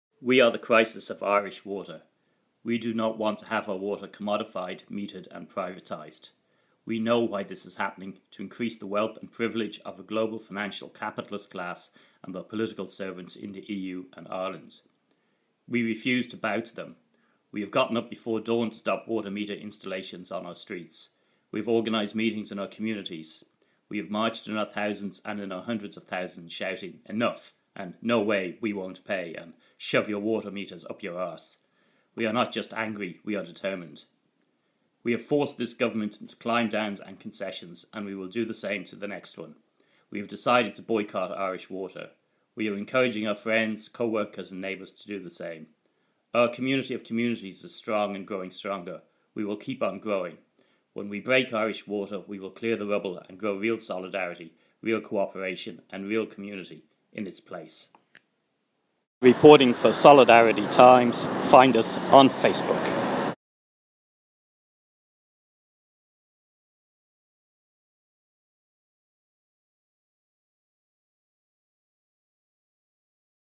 We are the Crisis of Irish Water
We are the crisis of Irish Water! We do not want to have our water commodified, metered, and privatised. We know why this is happening – to increase the wealth and privilege of a global financial capitalist class and their political servants in the EU and Ireland.

We refuse to bow to them. We have gotten up before dawn to stop water meter installation on our streets. We have organised meetings in our communities. We have marched in our thousands and in our hundreds of thousands, shouting “Enough!” and “No way, we won’t pay!” and “Shove your meters up your arse!” We are not just angry, we are determined.

We have forced this government into climb-downs and concessions, and we will do the same to the next one. We have decided to boycott Irish Water. We are encouraging our friends, co-workers and neighbours to do the same. Our community of communities is strong and growing stronger. We will keep on growing. When we break Irish Water, we will clear the rubble and grow real solidarity, real co-operation and real community in its place.